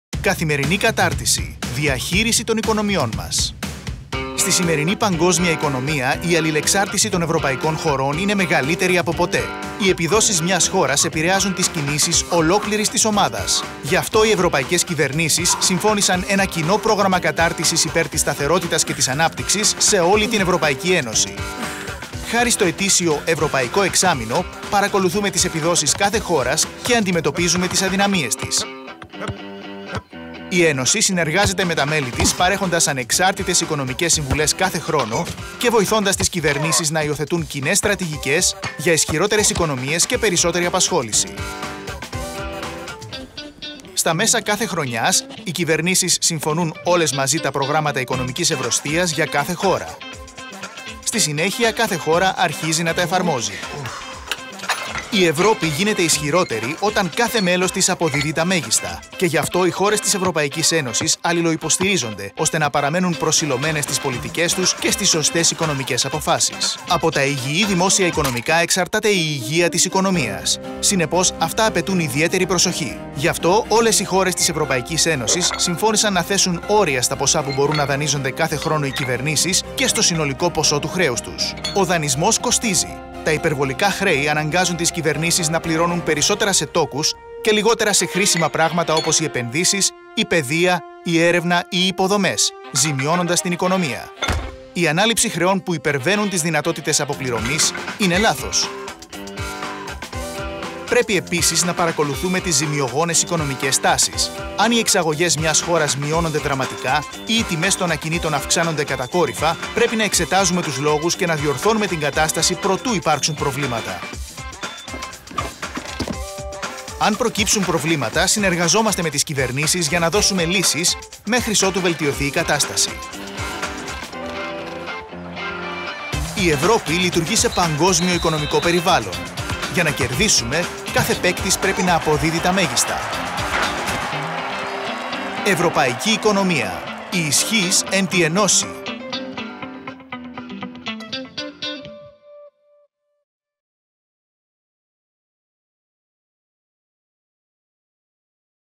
Καθημερινή κατάρτιση. Πώς να διαχειριζόμαστε τις οικονομίες μας
Η Ευρωπαϊκή Επιτροπή παρακολουθεί εκ του σύνεγγυς την οικονομική κατάσταση σε όλη την Ευρώπη. Βοηθά τις κυβερνήσεις να συντονίζουν τη δράση τους, έτσι ώστε όλοι να συνεργάζονται για το κοινό καλό, και δημοσιεύει ανεξάρτητες και τεκμηριωμένες αξιολογήσεις των εθνικών πολιτικών. Μάθετε πώς λειτουργεί.